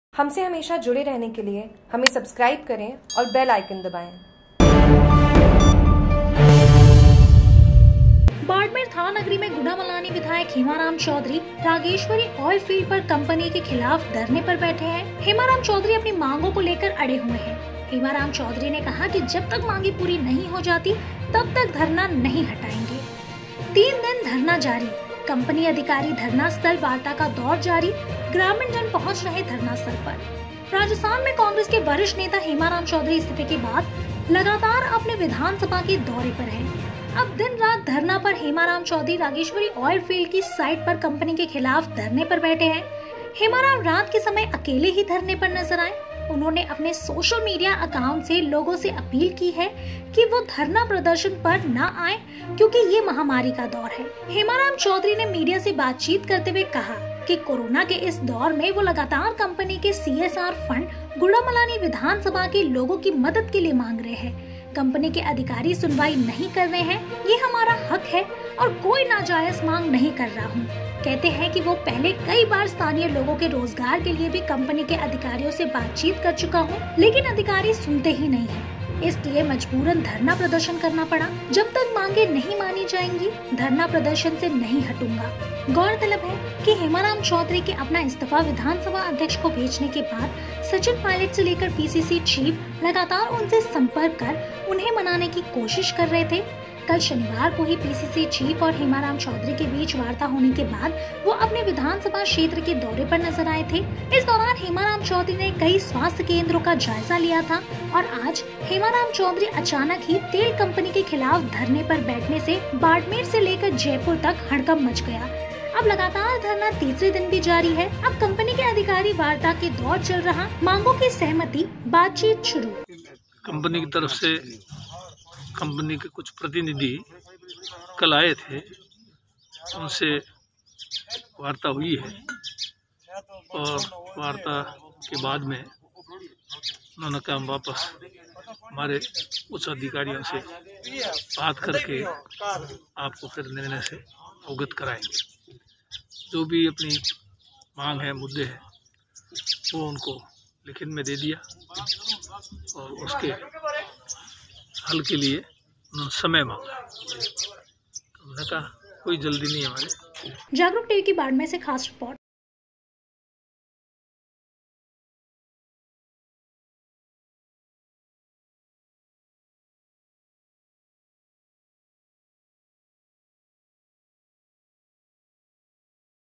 बाडमेर: हेमाराम चौधरी रागेश्वरी ऑयल फील्ड कंपनी के खिलाफ धरने पर बैठे हैं l
राजस्थान में कांग्रेस के वरिष्ठ नेता हेमाराम चौधरी इस्तीफे के बाद लगातार अपने विधानसभा के दौरे पर हैं. अब दिन रात धरना पर    हेमाराम चौधरी रागेश्वरी ऑयल फील्ड की साइट पर कंपनी के खिलाफ धरने पर बैठे हैं. हेमाराम रात के समय अकेले ही धरने पर नजर आएl

उन्होंने अपने सोशल मीडिया अकाउंट से लोगों से अपील की है कि वह धरना-प्रदर्शन पर न आएं क्योंकि यह महामारी का दौर है.हेमाराम चौधरी ने मीडिया से बातचीत करते हुए कहा कि कोरोना के इस दौर में वह लगातार कंपनी से सीएसआर फंड गुड़ामालानी विधानसभा के लोगों की मदद के लिए मांग रहे हैं. कंपनी के अधिकारी सुनवाई नहीं कर रहे हैं l

यह हमारा हक है और कोई नाजायज मांग नहीं कर रहा हूं. कहते हैं कि वह पहले कई बार स्थानीय लोगों के रोजगार के लिए भी कंपनी के अधिकारियों से बातचीत कर चुका हूं. लेकिन अधिकारी सुनते ही नहीं है. इसलिए मजबूरन धरना प्रदर्शन करना पड़ा l

जब तक मांगें नहीं मानी जाएंगी, धरना-प्रदर्शन से नहीं हटूंगा.गौरतलब है कि हेमाराम चौधरी के अपना इस्तीफा विधानसभा अध्यक्ष को भेजने के बाद सचिन पायलट से लेकर पीसीसी चीफ लगातार उनसे संपर्क कर उन्हें मनाने की कोशिश कर रहे थे. कल शनिवार को ही पीसीसी चीफ और हेमाराम चौधरी के बीच वार्ता होने के बाद वह अपने विधानसभा क्षेत्र के दौरे पर नजर आए थे. इस दौरान हेमाराम चौधरी ने कई स्वास्थ्य केंद्रों का जायजा लिया था और आज हेमाराम चौधरी अचानक ही तेल कंपनी के खिलाफ धरने पर बैठने से बाड़मेर से लेकर जयपुर तक हड़कंप मच गया अब लगातार धरने  तीसरे दिन जारी अब कँपनी के अघिकारी वार्ता के दौर चल रहा माँगे पर सहमति  बातचीत शुरू l